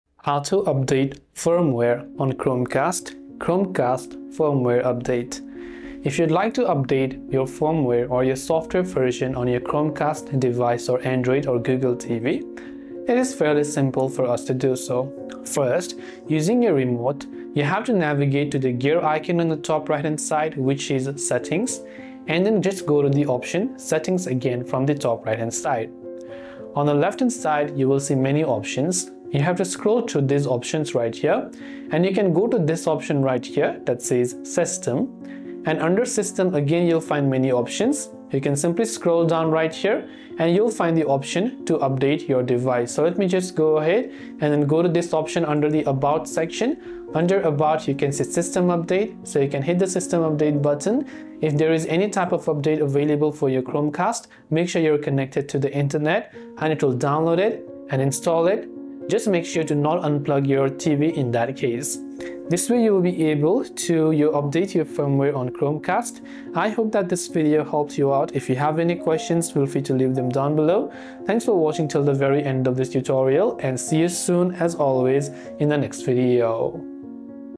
How to Update Firmware on Chromecast
If you need to update the firmware on your Chromecast device, Android TV, or Google TV, the process is simple and ensures your device runs smoothly with the latest features and security improvements. Chromecast usually updates automatically when connected to the internet, but you can manually check and install updates if needed.

To begin, ensure your Chromecast is connected to a stable Wi-Fi network. Using your remote, navigate to the settings menu by selecting the gear icon in the top-right corner of your screen. Once inside settings, scroll through the available options and select "System."

Under the "System" section, locate the "About" option. Inside the "About" menu, you will find the "System Update" option. Click on it to check for any available firmware updates. If an update is found, ensure your Chromecast remains connected to power and Wi-Fi while it downloads and installs. Do not unplug your TV or Chromecast during this process, as interruptions could cause issues with the update.

Keeping your Chromecast updated helps improve performance, fix bugs, and provide access to new features. If your device does not update, you may need to restart it, check your internet connection, or perform a factory reset if necessary.

Timestamps:
0:00 Introduction
0:06 Navigating to Chromecast settings
0:32 Finding the system update option
0:48 Updating Chromecast firmware
1:03 Conclusion